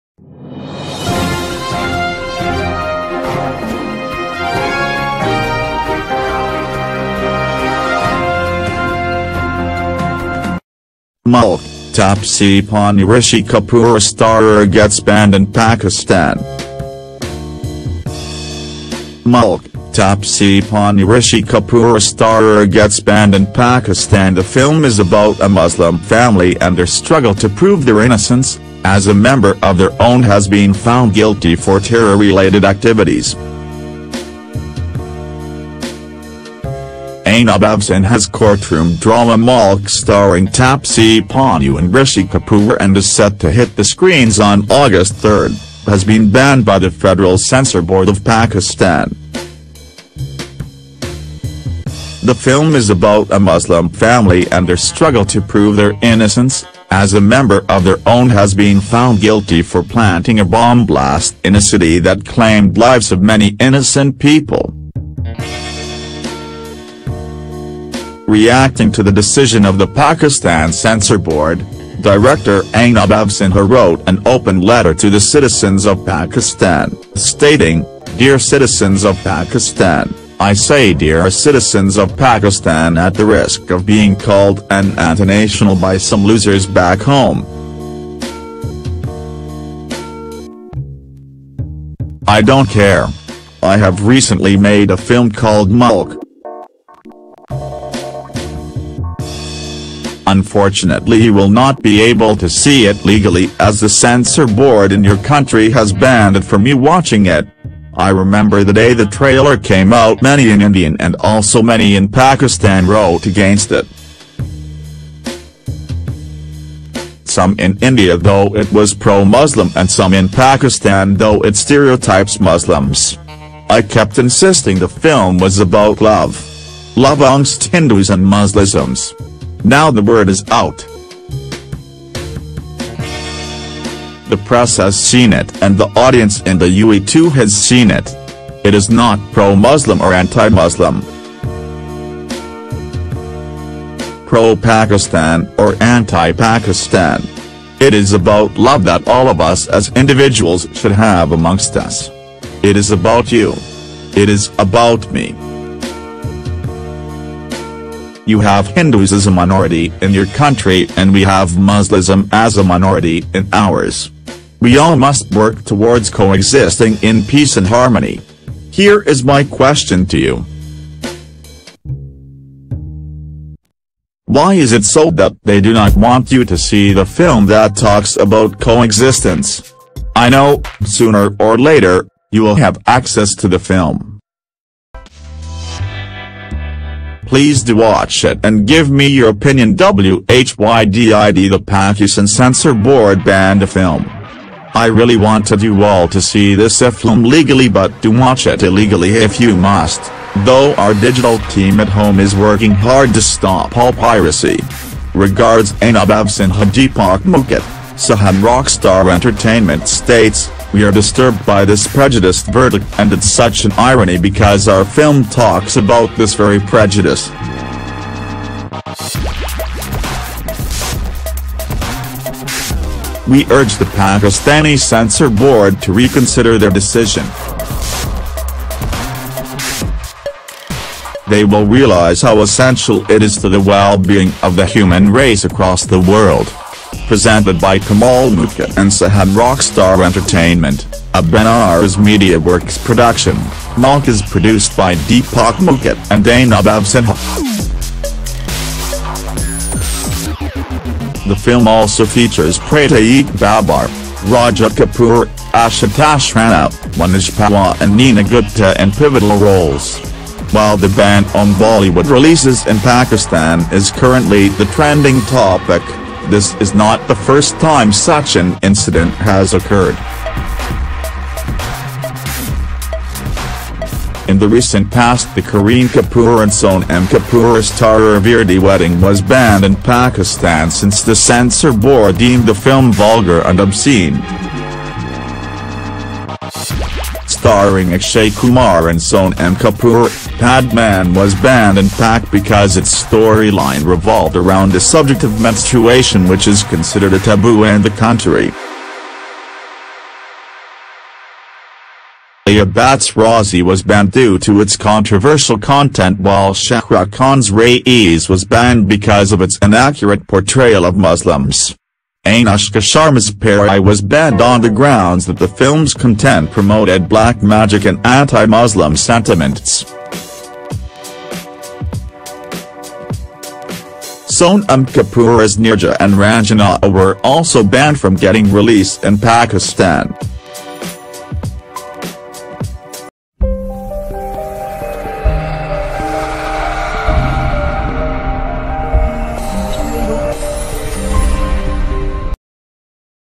Mulk: Taapsee Pannu-Rishi Kapoor starrer gets banned in Pakistan
The film is about a Muslim family and their struggle to prove their innocence, as a member of their own has been found guilty for terror-related activities.